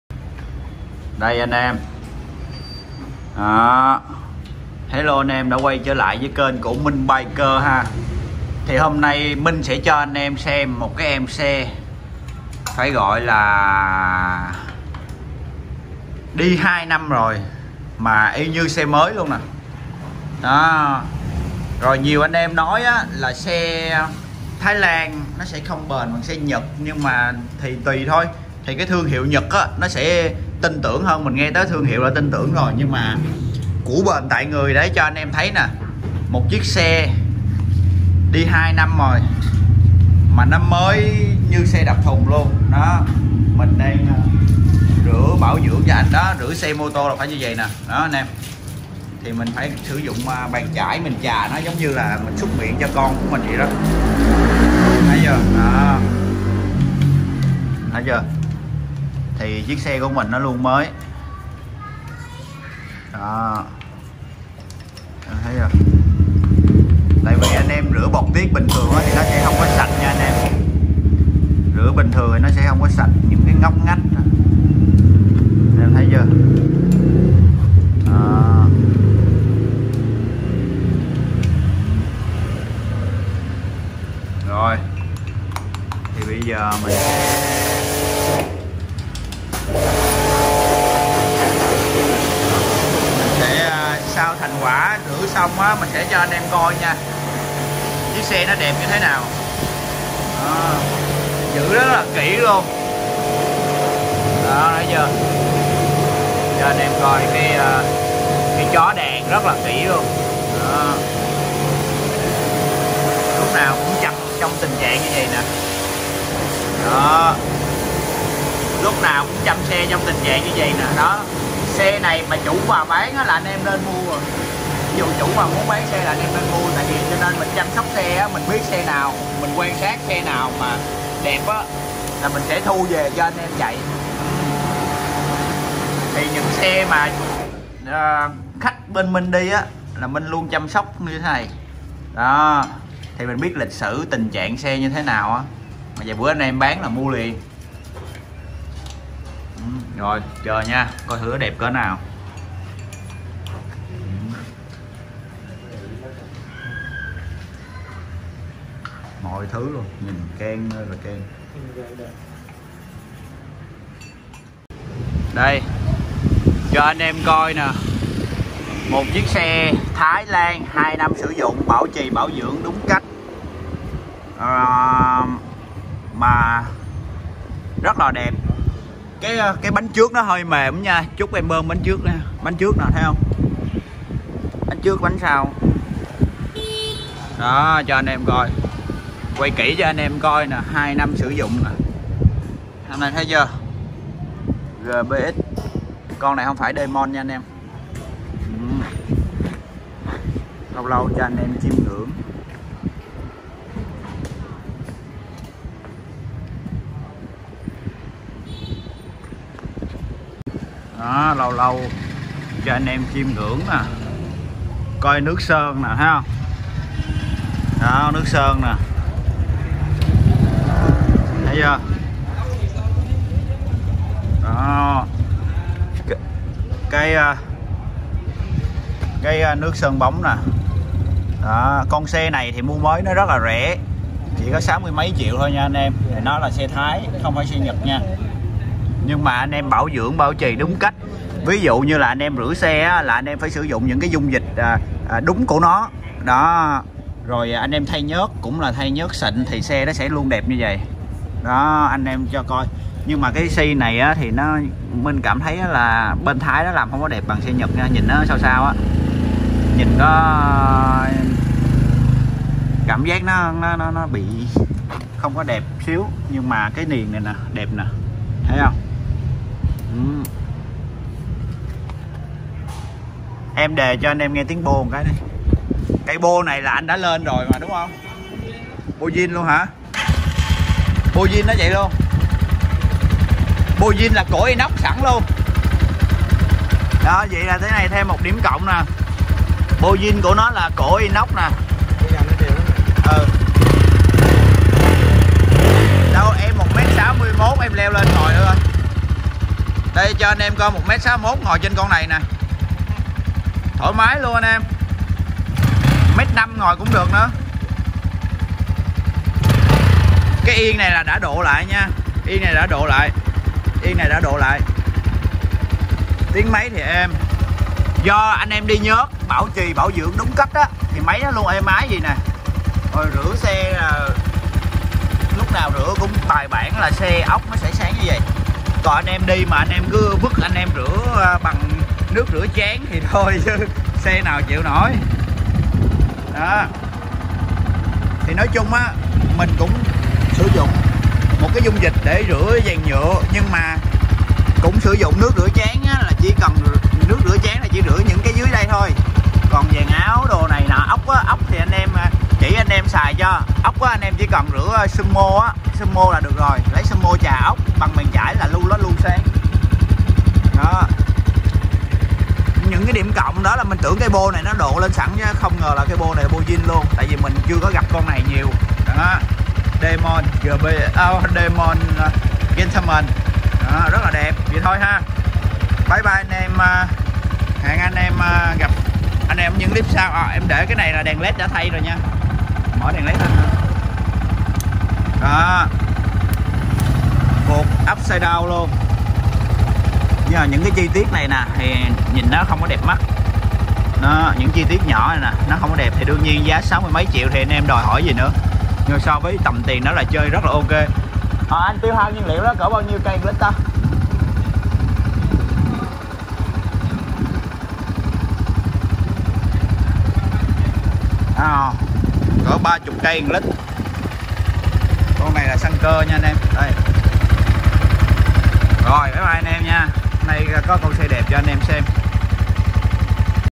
Của Bền Tại Người - Cách Cho xe Moto Luôn Mới Sau Hai Năm | MinhBiker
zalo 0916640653
địa chỉ 25 lô C lý Chiêu Hoàng Phường 10 Quận 6